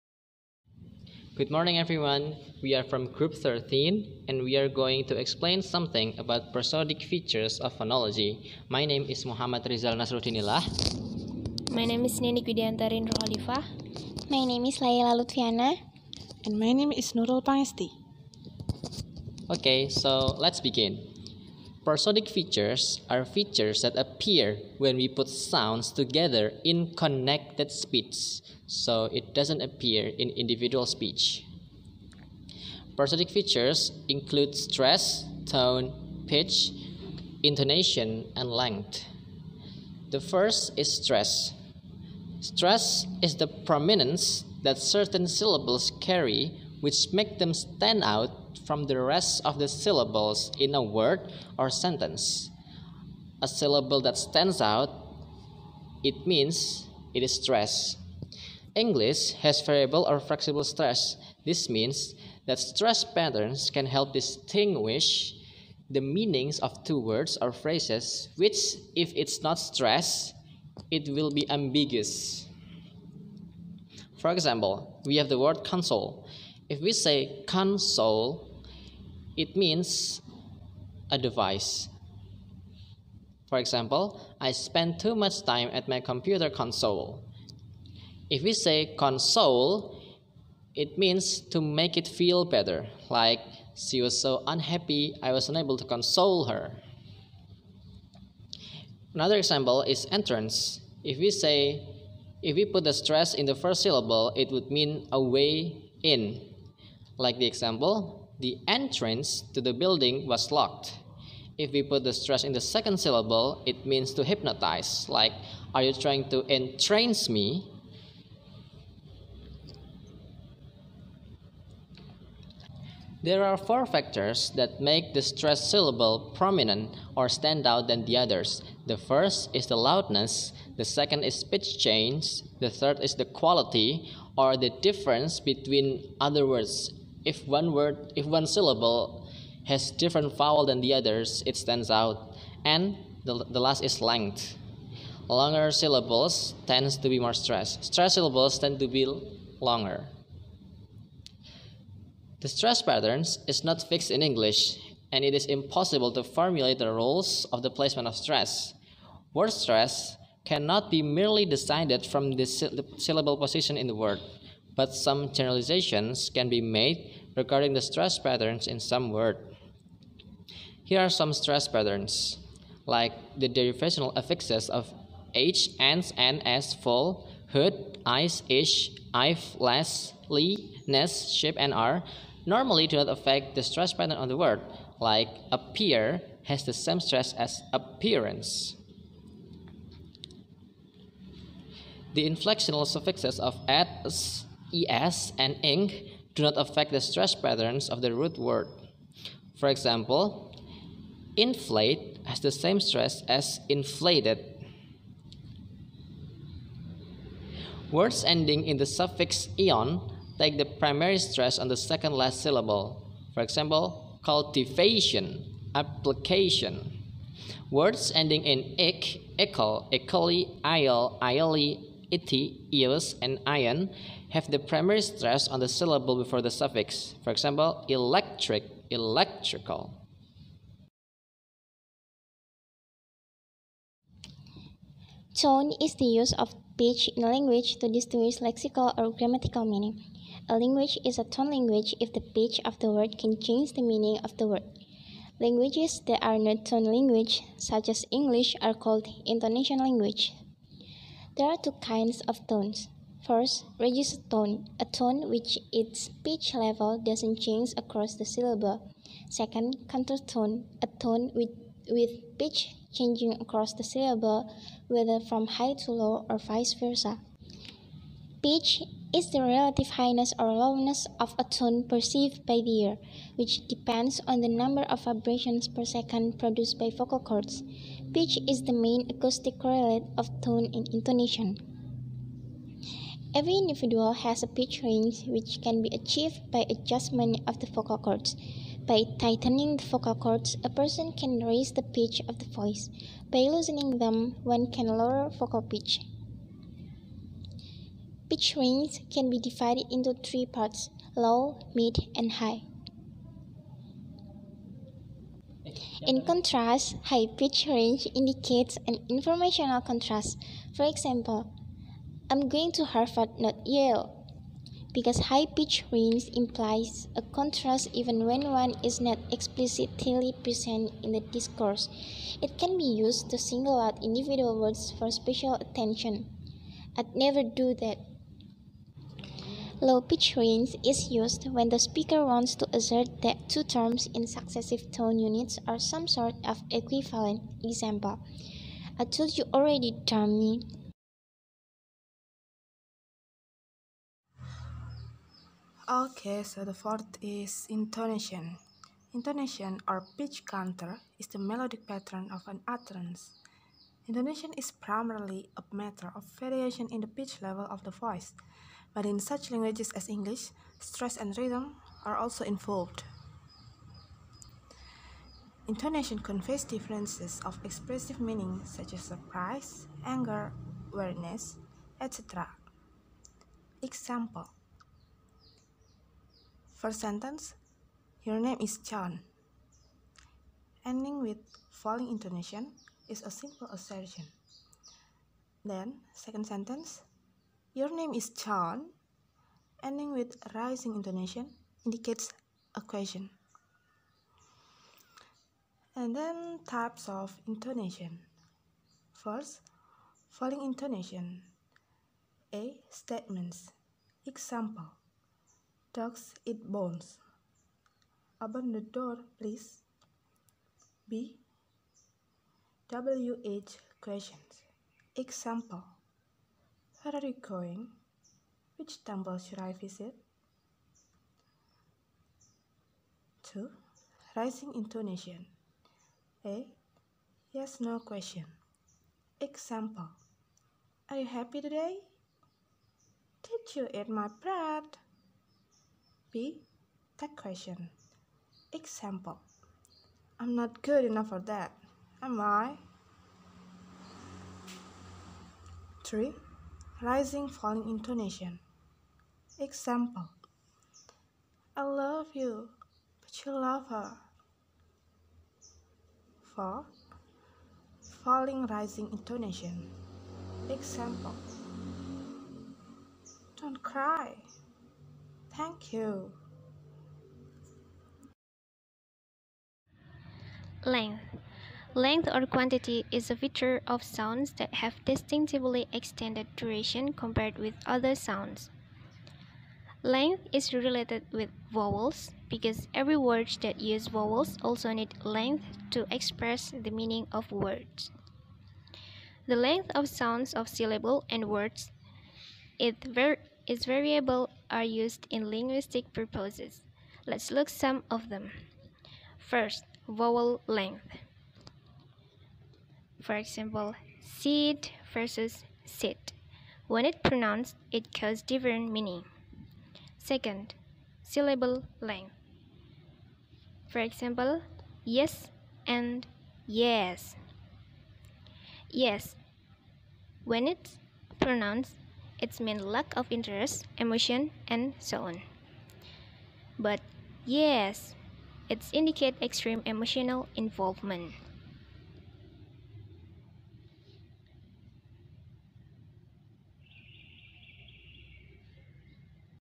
Prosodic Features - Phonetics and Phonology
A presentation about Prosodic Features of Phonology

Group Members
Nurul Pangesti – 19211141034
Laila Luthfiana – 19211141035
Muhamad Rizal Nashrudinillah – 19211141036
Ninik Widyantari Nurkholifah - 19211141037